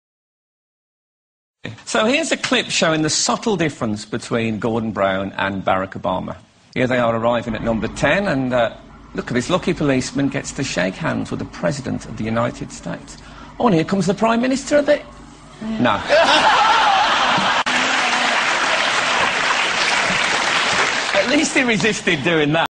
MUST WATCH: Check out Difference Between American and British ( Barack obama and Gordan Brown )
CHECK OUT WATS THE DIFFERENCE BETWEEN AN AMERICAN AND A BRITISHER. THIS IA REALLY FUNNY VIDEO SO DON'T FORGET TO CHECK THIS OUT.
ENJOYYYYYYYYYYYY.